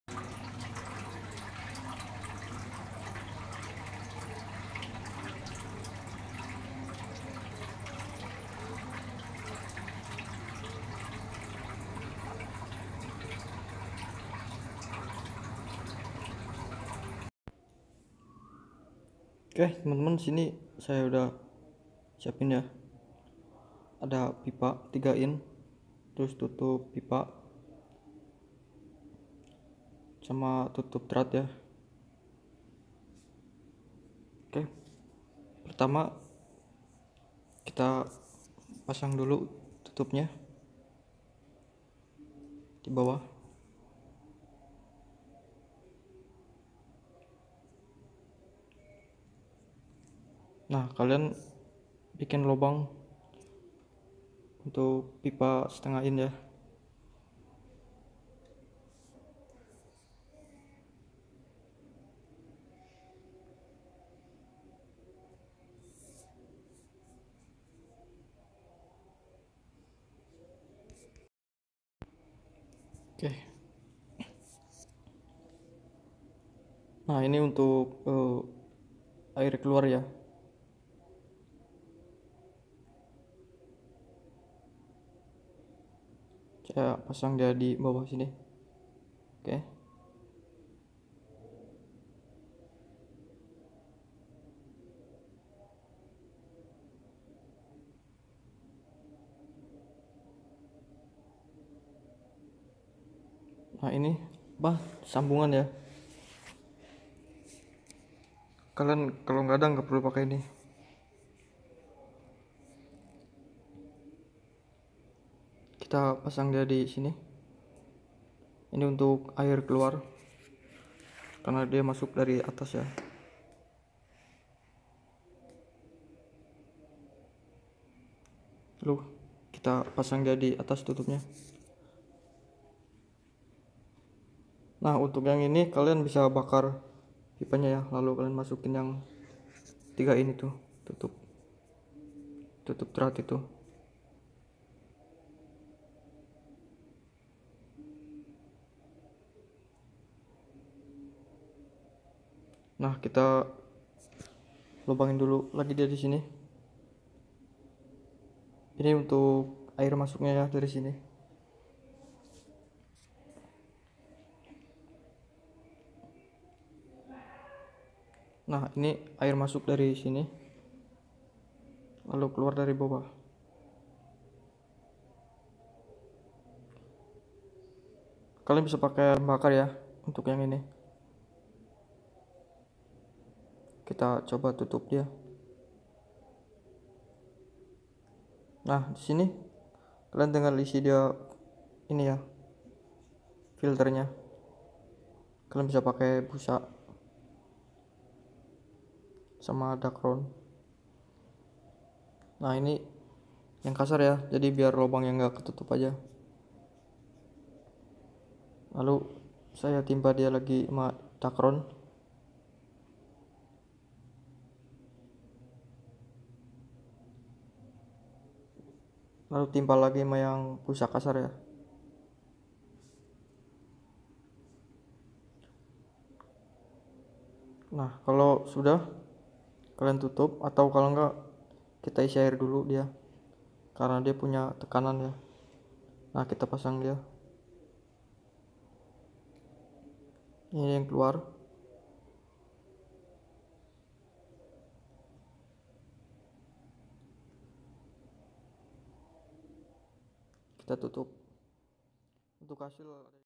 Filter aquarium dari paralon bekas how to make aquarium filter part 2
hallo teman teman nah kali ini saya membuat filter dari paralon lagi ya
dan saya coba untuk lebih simpel lagi
berikut bahan bahannya
1 pipa 3inch
lebih besar lebih bagus
2 knop/ tutp pipa 3inch
3 tutup drat pipa 3inch
Selebihnya kalian bisa cek dividio ya